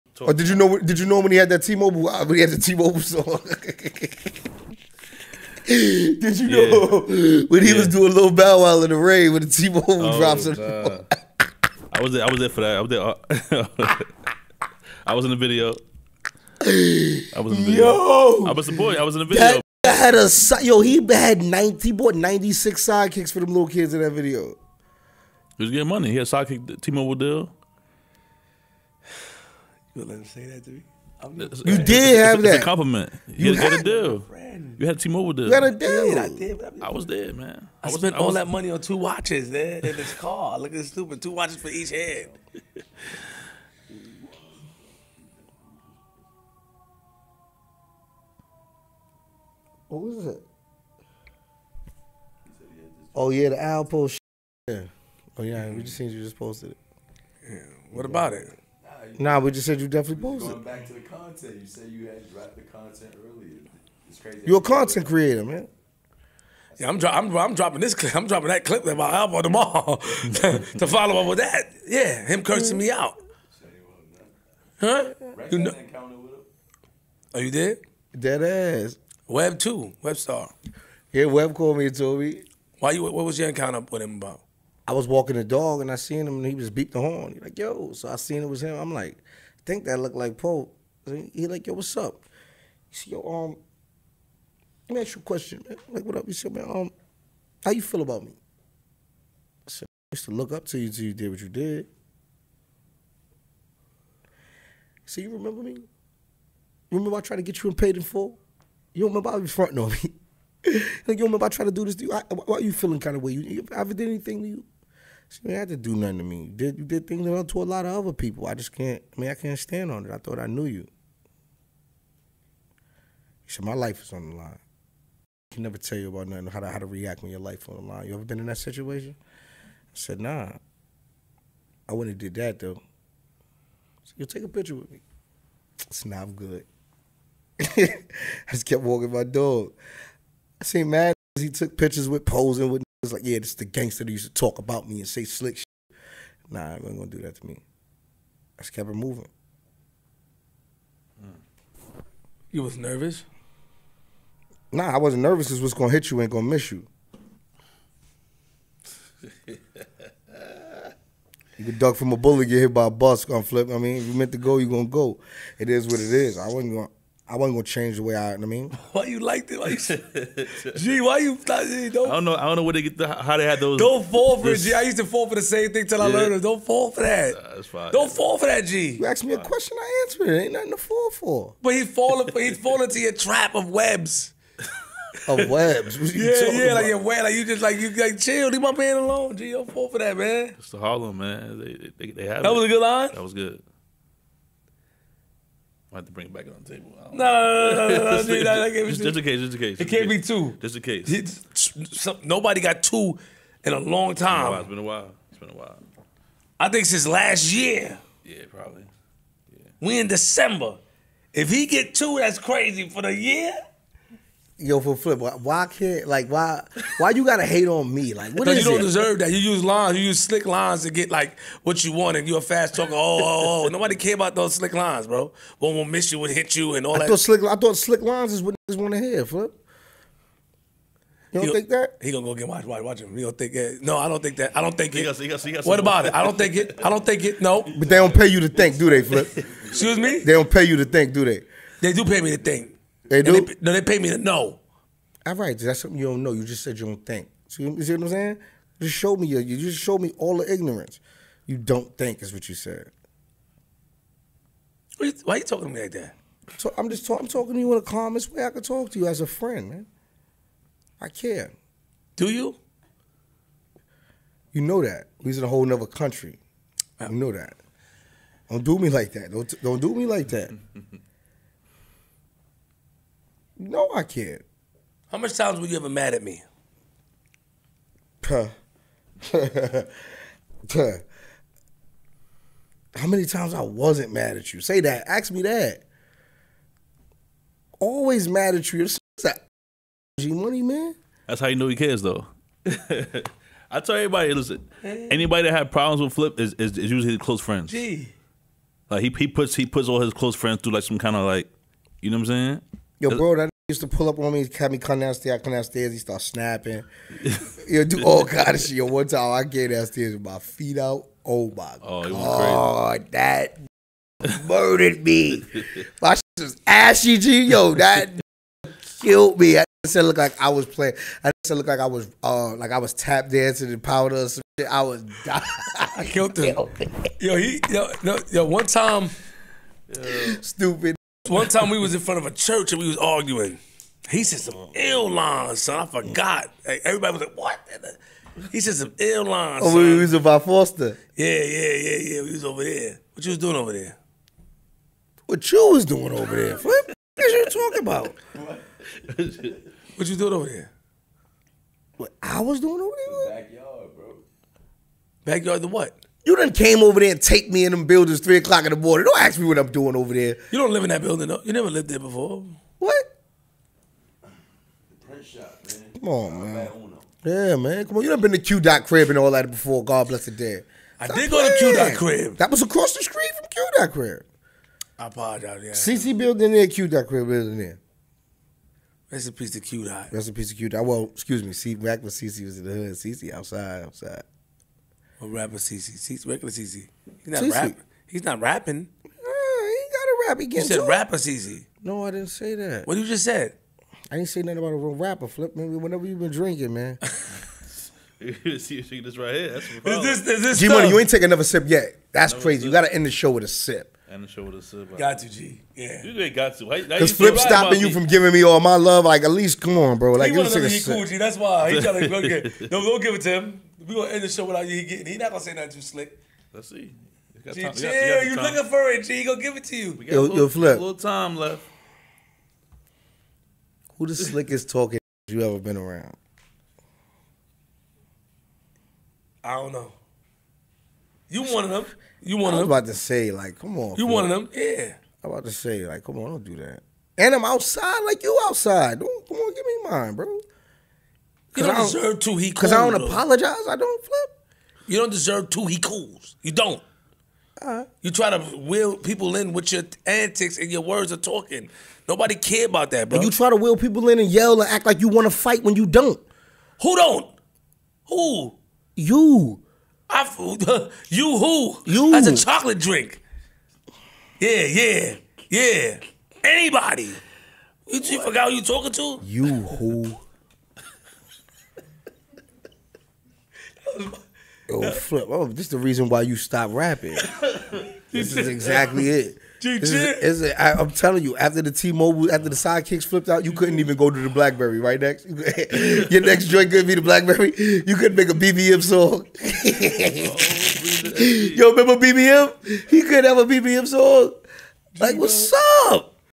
“ALPO PRESSED ME OUT & TRIED TO TAKE A PICTURE WITH ME” FLIP & REX GO AT IT - F.D.S #220 T-REX
On The 220th Episode Of FlipDaScript The DUO sits down again with BATTLE RAP LEGEND !! TREX & for 2 hours they touch on plenty of topics, The Past, K shine, Murda Mook,, Rex Health, Turning himself in & much more - BUT IN THIS CLIP - Rex tells a story about Alpo seeing him outside one morning when he was walking his dog, Rex also talks about how PO tried to take a picture with him & Rex declined. Flip & Rex Go at it again about flips Tmobile deal